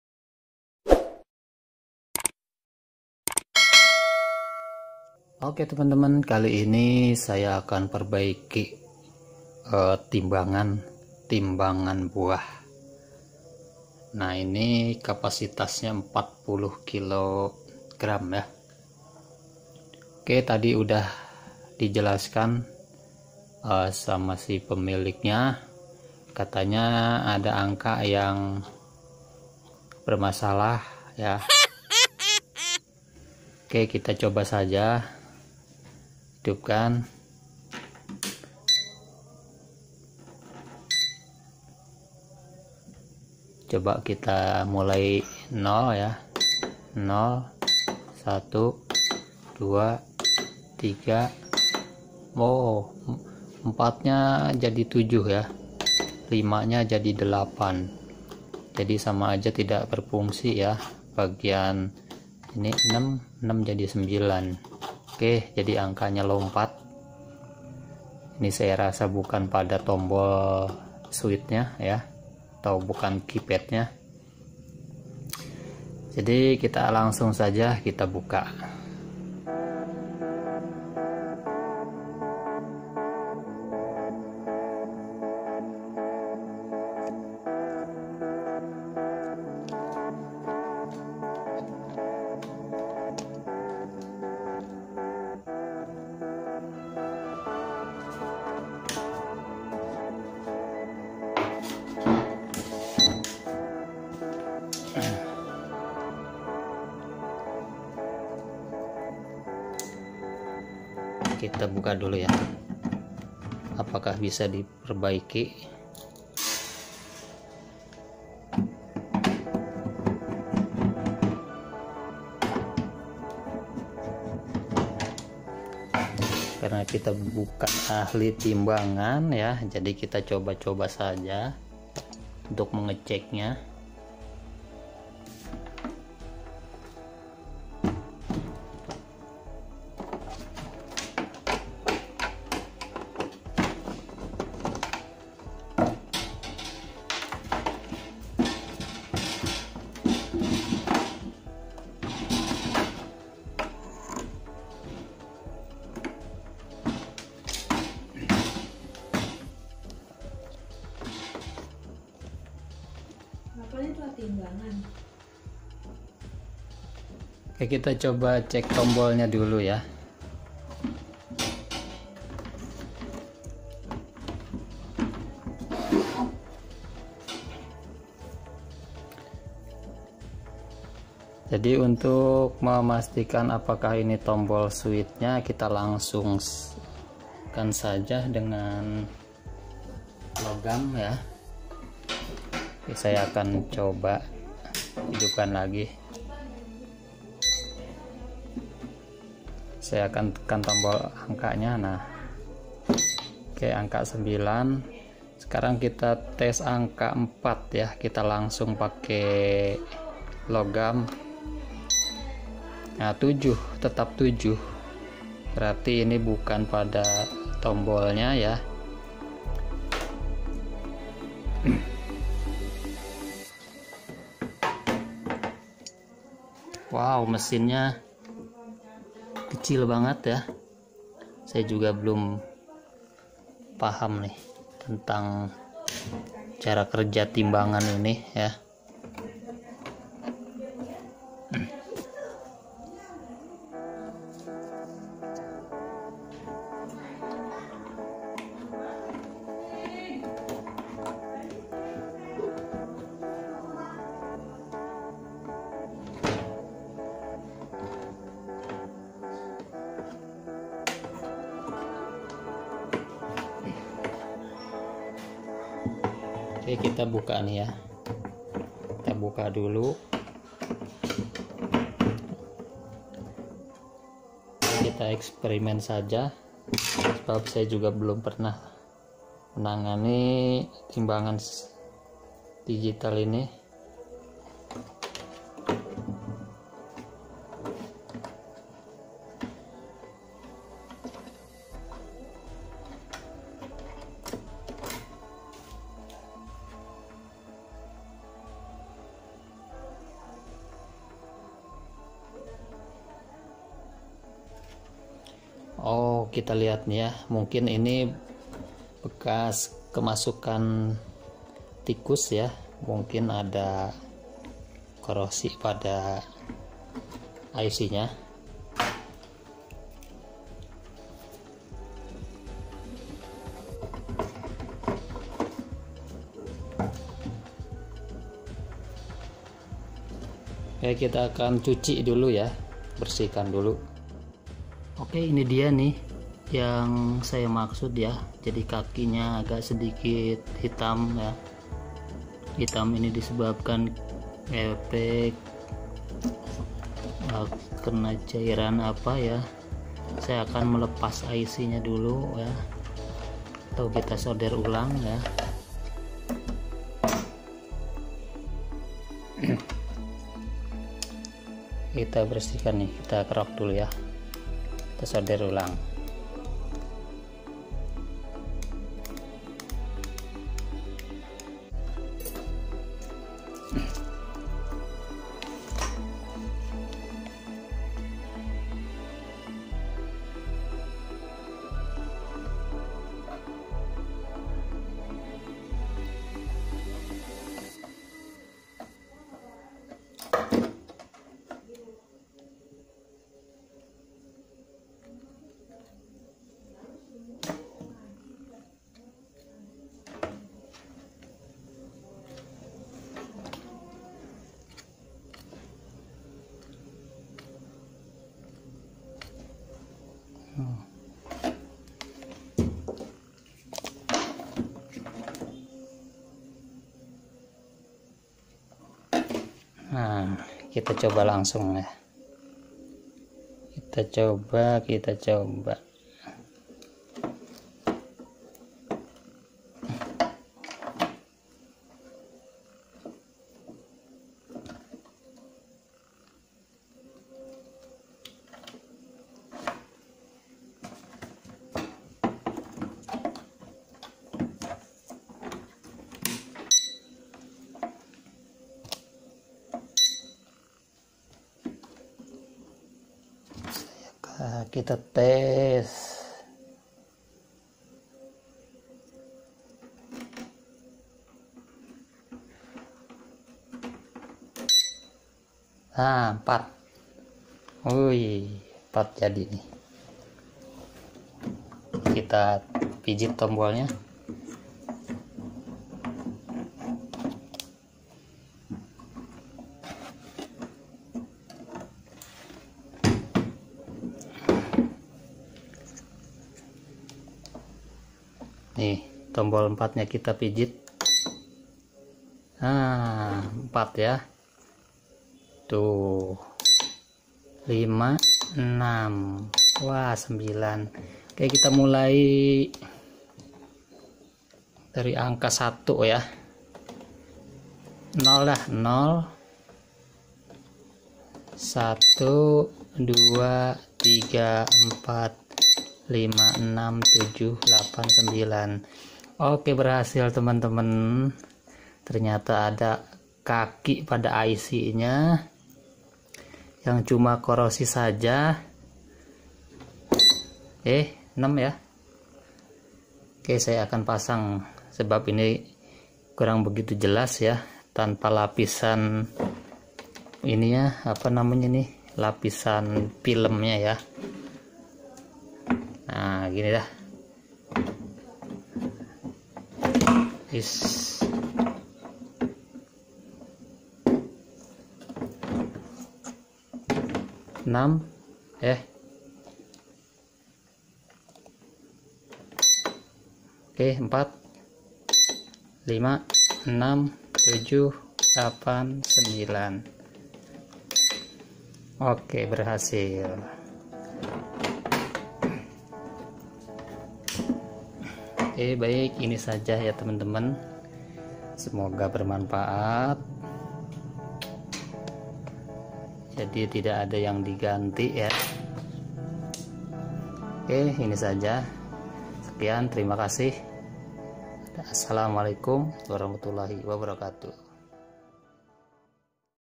VLOG # CARA PERBAIKI TIMBANGAN DIGITAL ERROR 3 DIGIT KEYPAD 4 5 6
Vlog perbaiki timbangan digital error 3digit pada keypad, Bukan rusak tombol keypad ny efek kena cairan jadi pin ic korosi. Alhmdullh bisa diatasi